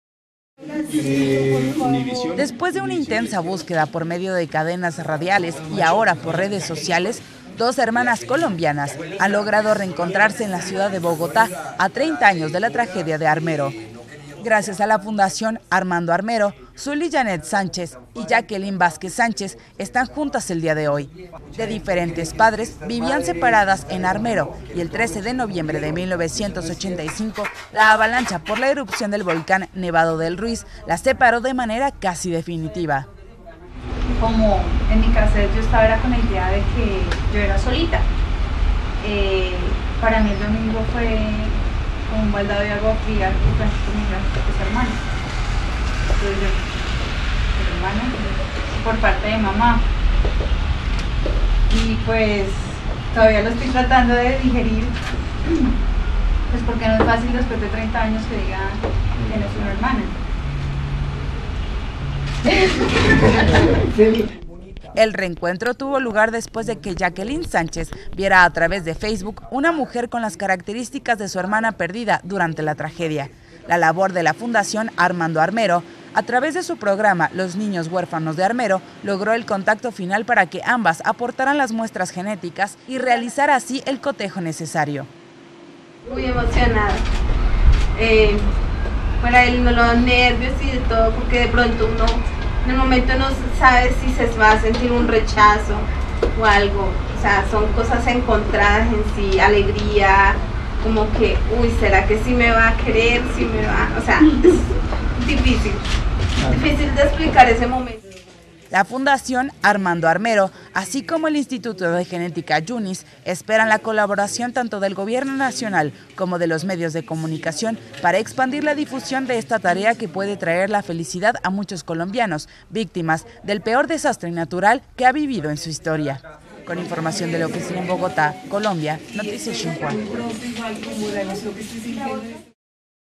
Hermanas se reencuentran tras 30 años de la tragedia de Armero
Después de una intensa búsqueda por medio de cadenas radiales y ahora por redes sociales, dos hermanas colombianas han logrado reencontrarse en la ciudad de Bogotá, a 30 años de la tragedia de Armero